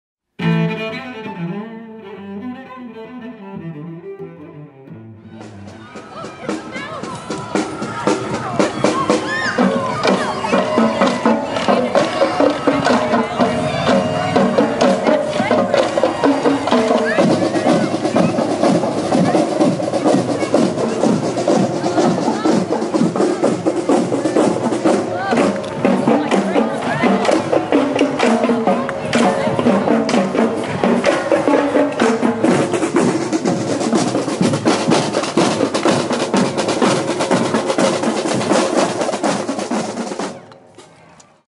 The Bay Rat
The Bay Rat and The Bay Ratz Marching Battery at The Mystic Krew of the Seahorse parade in Bay St. Louis, Mississippi. Lundi Gras 2020